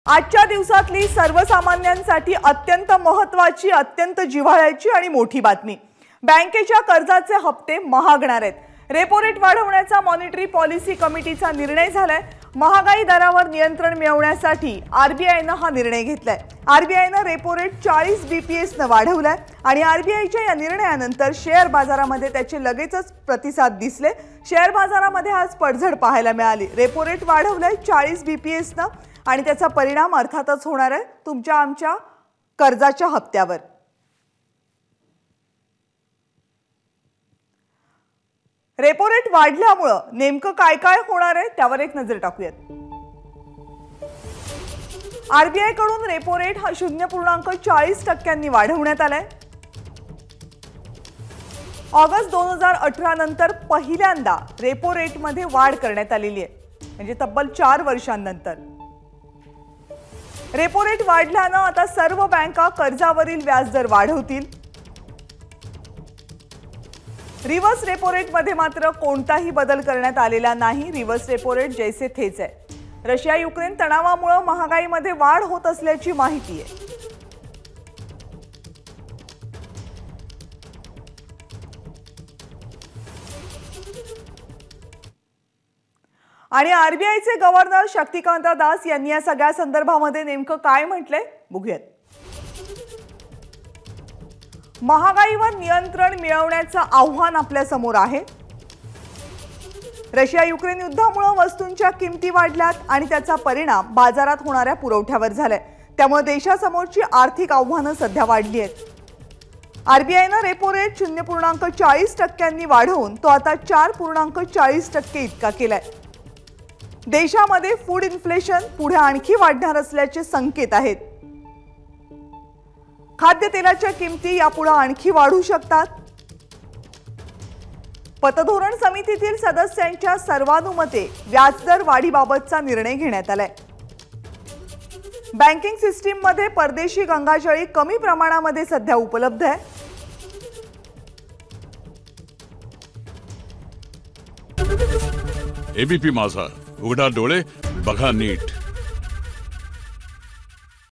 RBI: रेपो रेट वाढल्याने काय होणार? मॉनिटरी पॉलिसी कमिटीचा निर्णय ABP Majha
रेपो रेट वाढवण्याचा मॉनिटरी पॉलिसी कमिटीचा निर्णय. महागाई दरावर  नियंत्रण मिळवण्यासाठी आरबीआयचा निर्णय